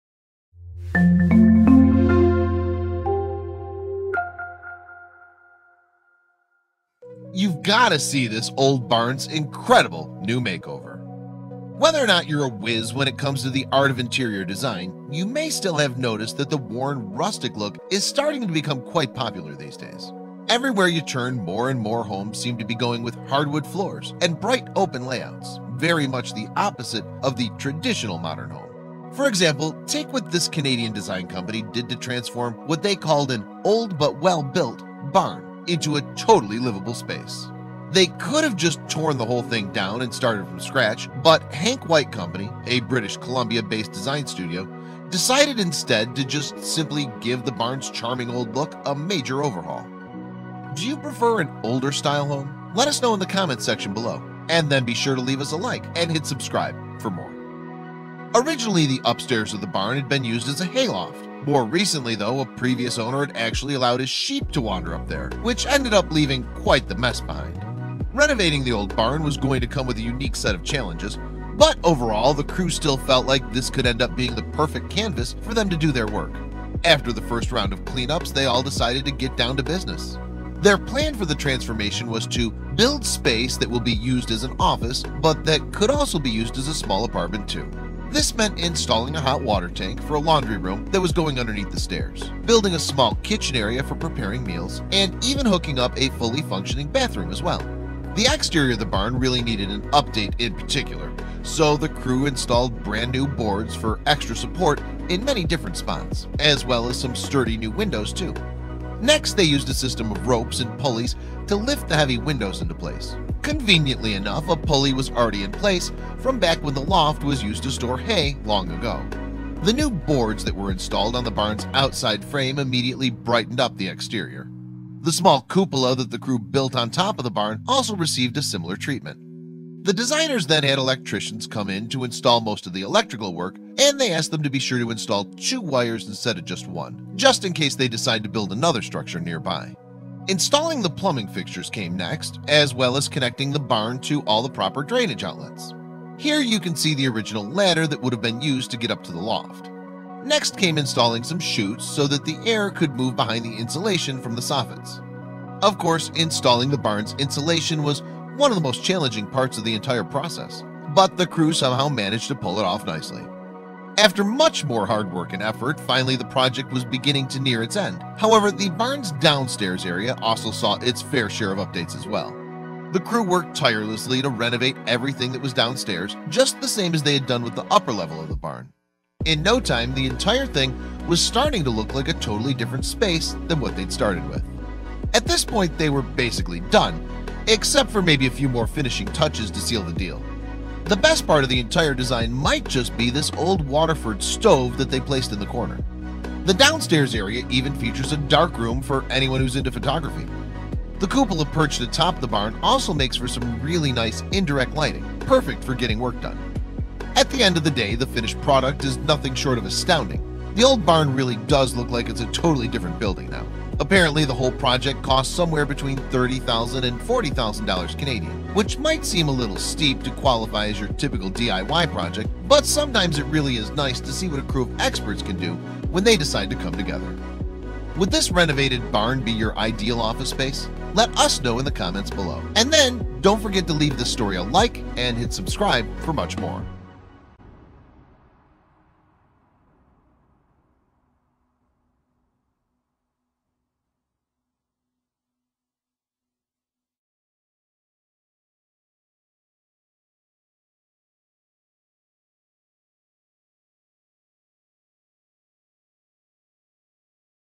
Genius Business Partners Transform An Old Barn Into An Office Space For The Ages
Whether you’re an interior design wiz or not, you may have noticed that the rustic, worn look is pretty popular these days. It seems like hardwood floors are more popular than ever!

Perhaps the best example of this trend is what this Canadian design company did to an “old but well built barn” that they found on a recently purchased property. Instead of tearing it down, or sprucing it up just a tad, they completely transformed it. The results speak for themselves…